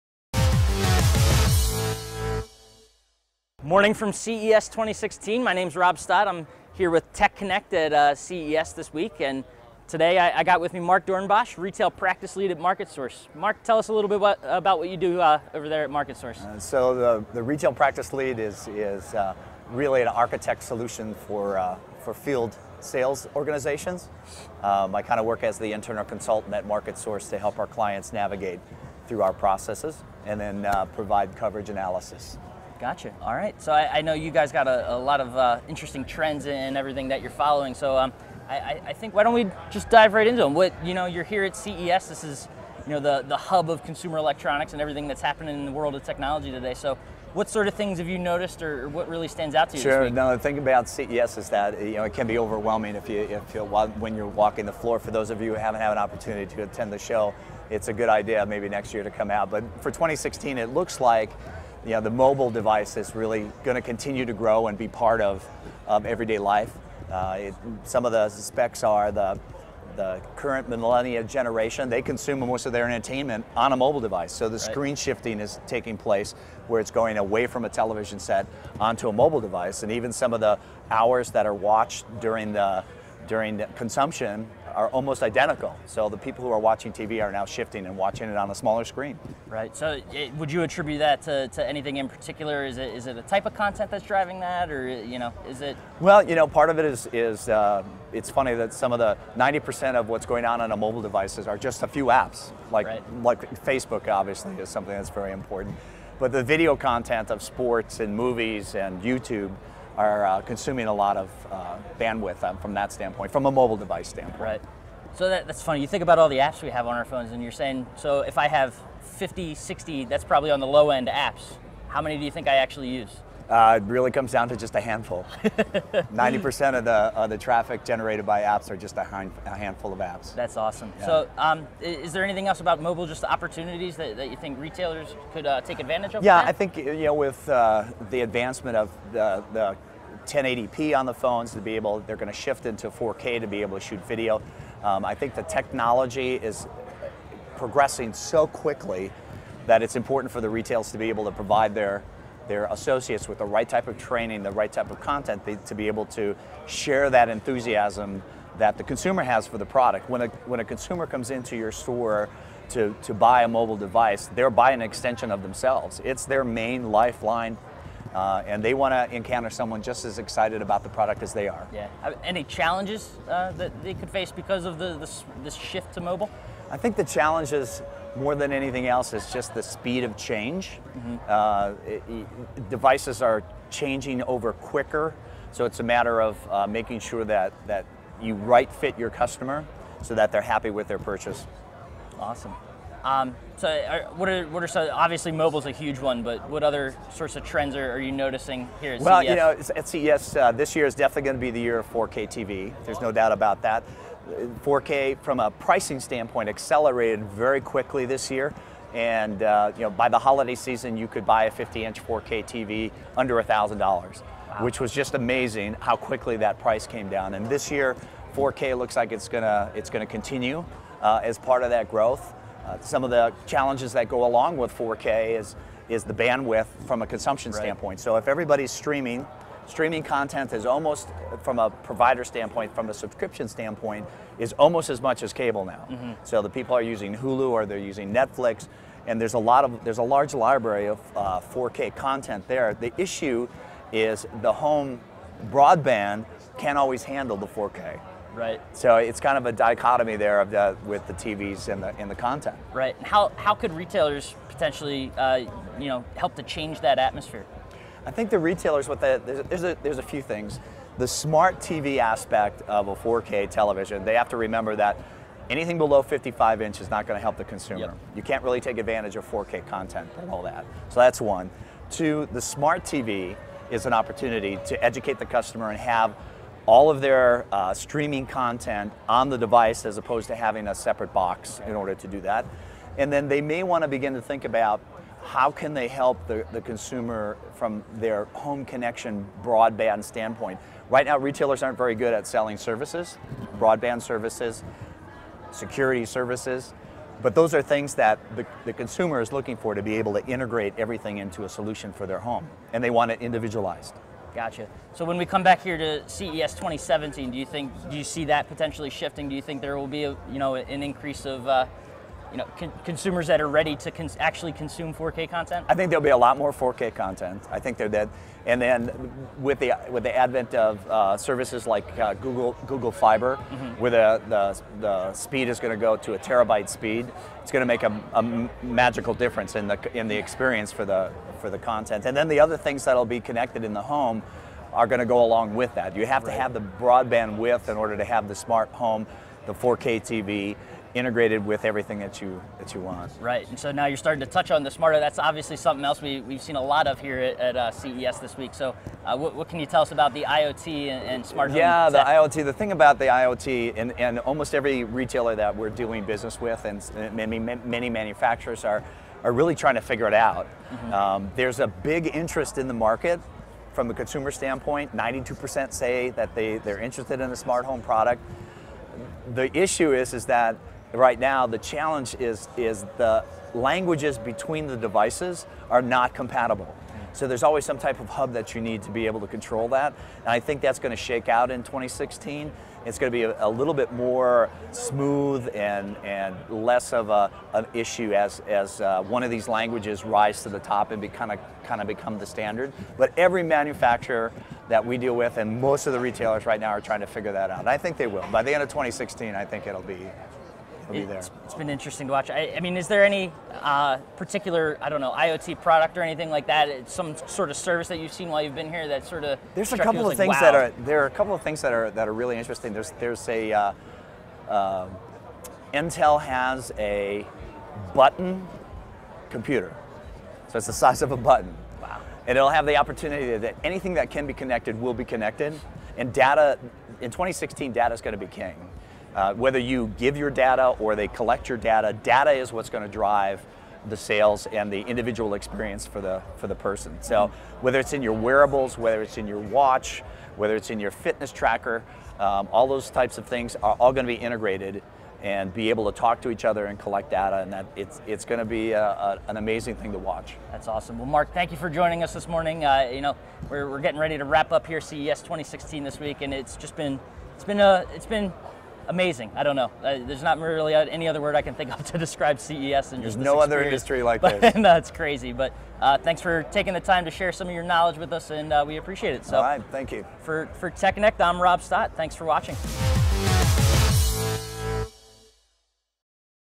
TechConnect Live from CES 2016 MarketSource Trend Report
MarketSource Practice Lead Mark Doornbosch interviewed at CES 2016